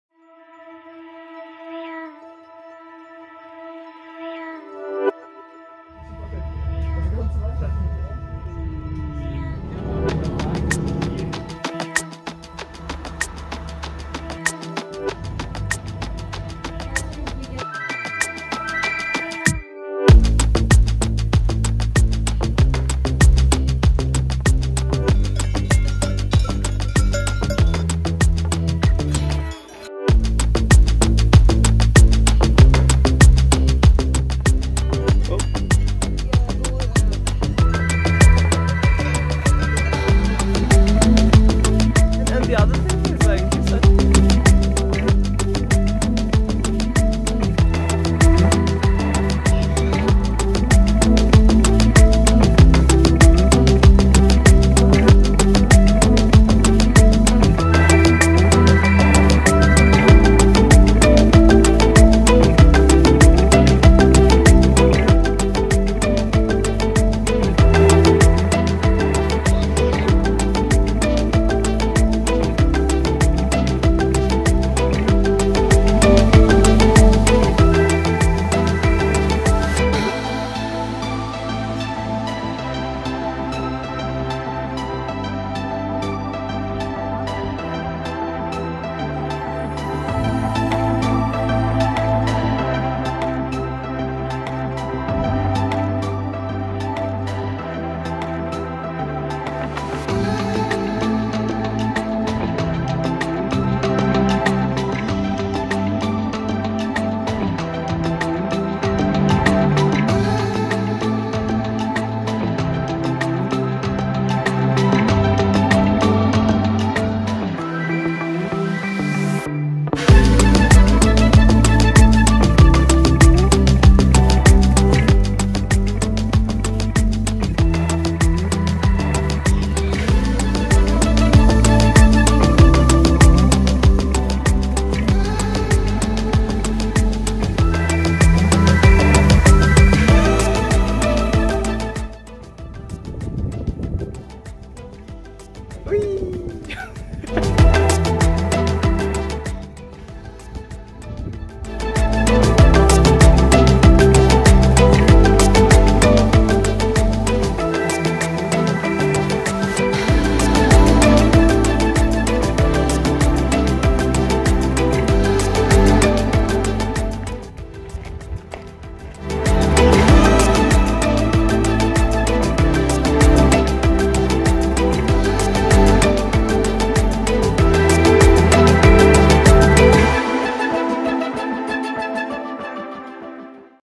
THE PANORAMIC STOOS HIKE, SWITZERLAND EP1
A beautiful warm summer hike full of endless panoramic views in Stoos Klingenstock-Fronalpstock, 🇨🇭 ❤

Famously known as the "Stoos Ridge Hike"!

Stoos, Switzerland ❤️ 🇨🇭

Summer ~
August, 2021

Captured on Samsung Galaxy S10e